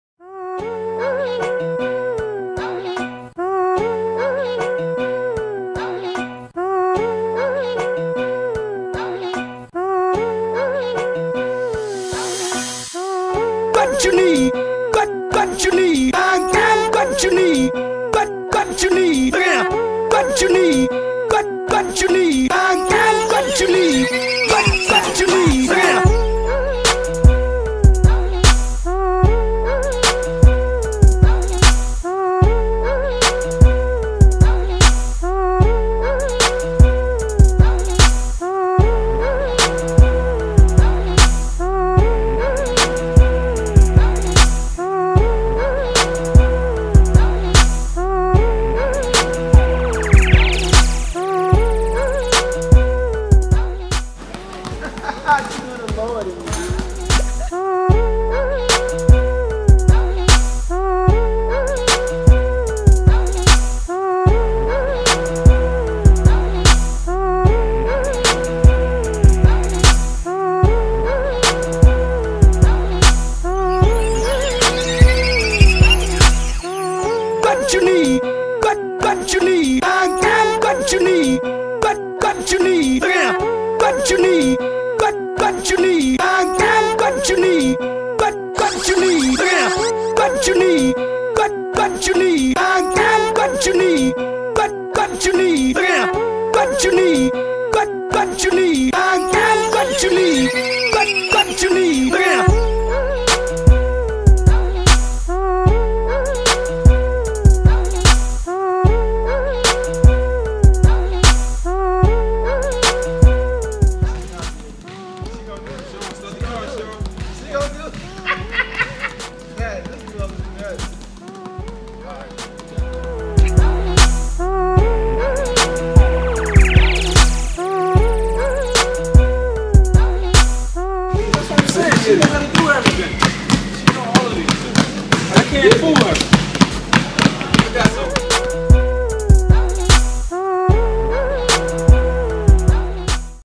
Pat The Roc Dribble Horse with 5th Grader Deja Kelly
Deja Kelly attended a Pat The Roc Ball Handling Academy in Austin, Texas...afterward, he challenged her to Dribble Horse!!!...she stepped up to the challenge...to say the least! :)

Facebook.com/PatTheRoc
Twitter.com/PatTheRoc